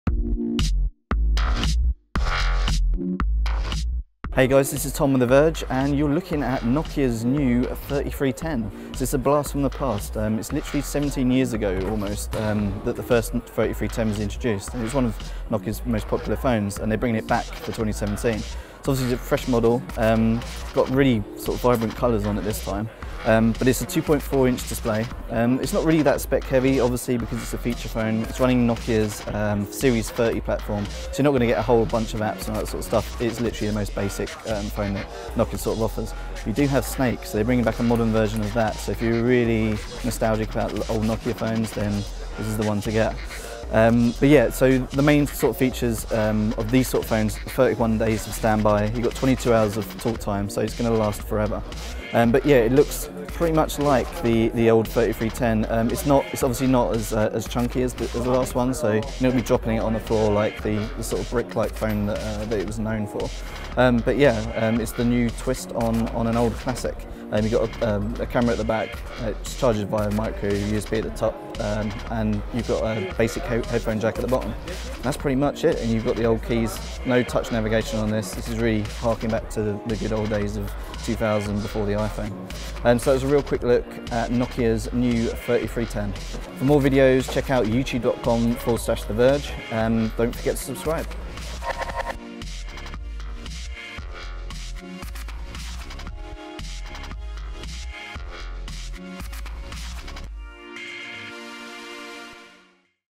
The Nokia 3310 is back
The Nokia 3310 is making a nostalgic return in the form of a more modern variant, thanks to Nokia-branded phone maker HMD. Like its predecessor, it will still be called the Nokia 3310, but this time it’s running Nokia’s Series 30+ software, with a 2.4-inch QVGA display, a 2-megapixel camera, and even a microSD slot.